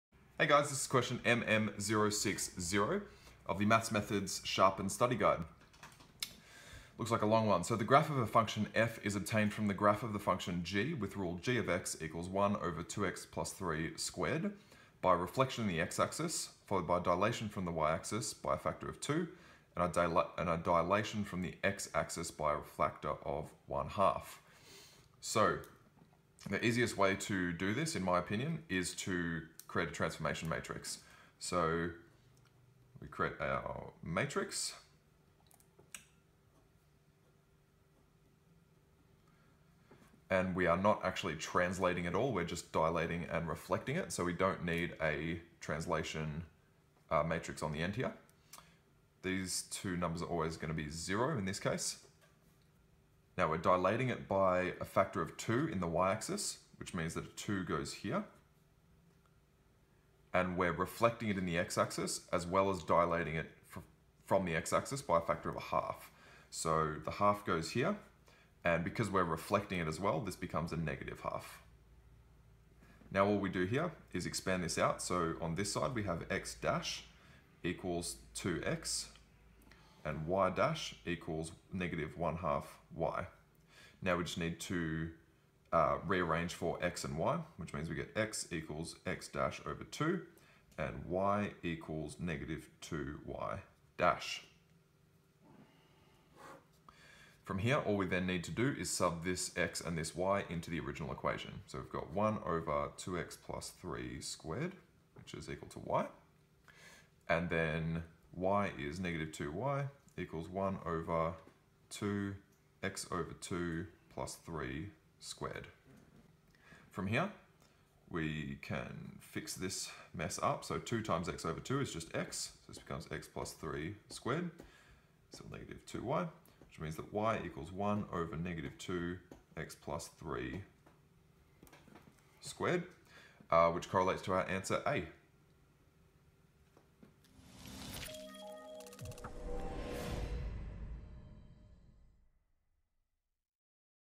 MM060 Maths Methods Fortify Study Guide - "How to apply dilations to a function"
This online video solution has been created specifically for the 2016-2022 FORTIFY: VCE Mathematical Methods Units 3/4 Study Guide.